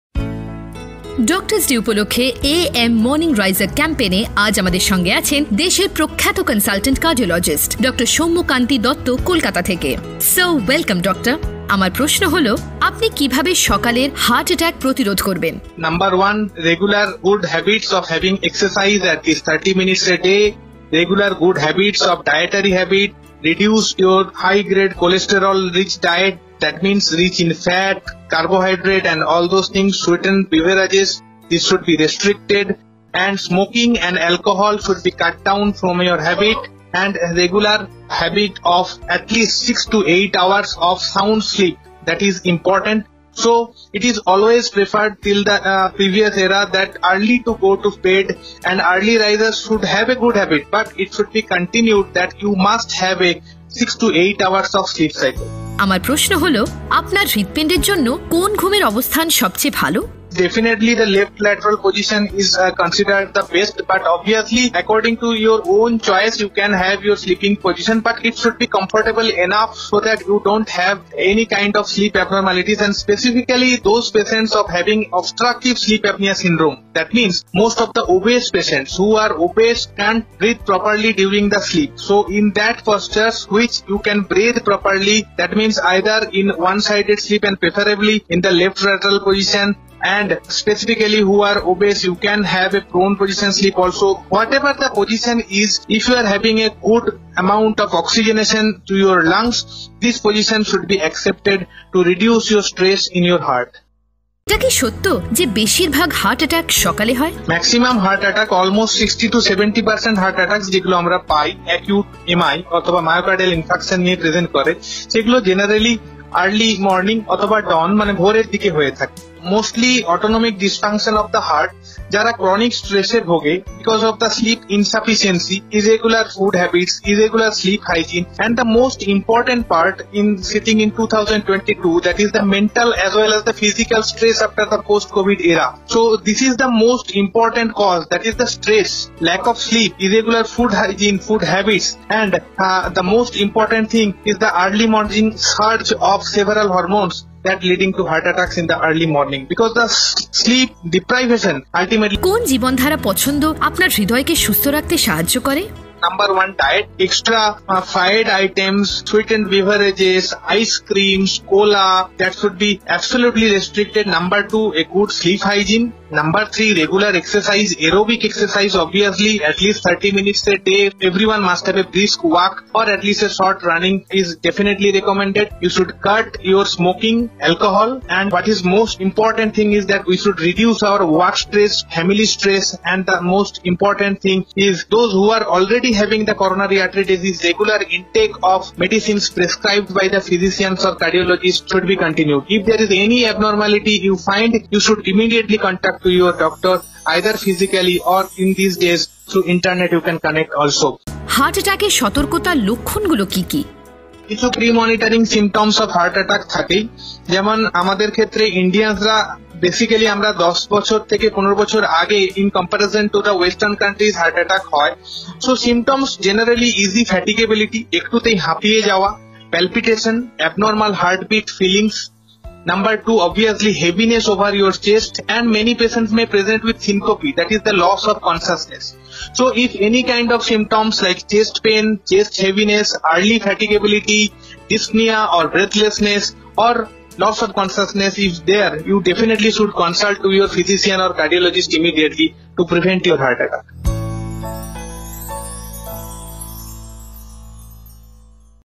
Dr. Sumya Kanti Dutta Cardiologist |Heart Diseases | Obesity |Cholesterol | Artery .
When one feels chest pain extending from the chest to the back and left arm, it could be the pain of a heart attack or angina. Pain should not be ignored and patients should go to the hospital immediately. In a heart attack, the common symptoms are breathlessness, palpitations, excessive sweating, severe chest pain, vomiting, and dizziness. After seeing such symptoms, patients should immediately call the medical service or go to the nearest hospital.

Dr. Sumya Kanti Dutta Consultant Interventional Cardiologist from Kolkata explains the causes and symptoms of heart attack and advises patients not to ignore any such pain.

To know more about AM Morning Riser and its facilities.